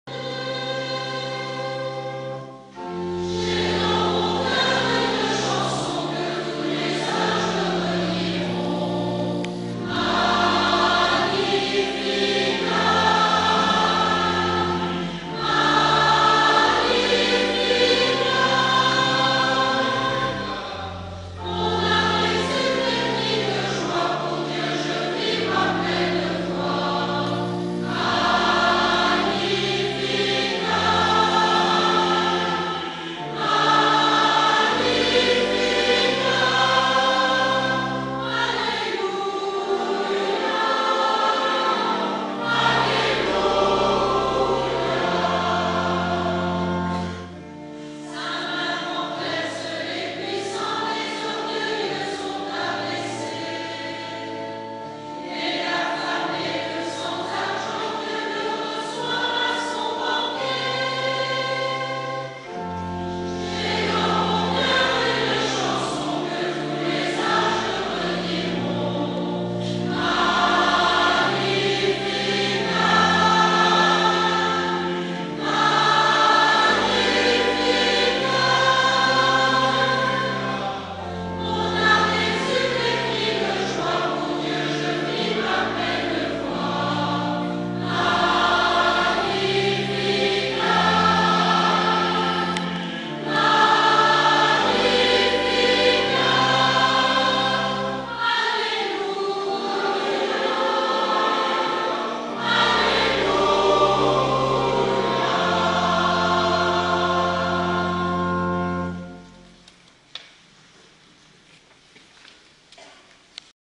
Chant d ' action de grâce, Ascension. Marquise 17 Mai 2012
Chant d 'action de grâce lors de la messe de bénédiction de l' orgue datant de 1862 , restauré ,Messe de l' Ascension Marquise Pas de Calais France ,17 Mai 2012
Ce hant s'inspire des paroles du magnificat.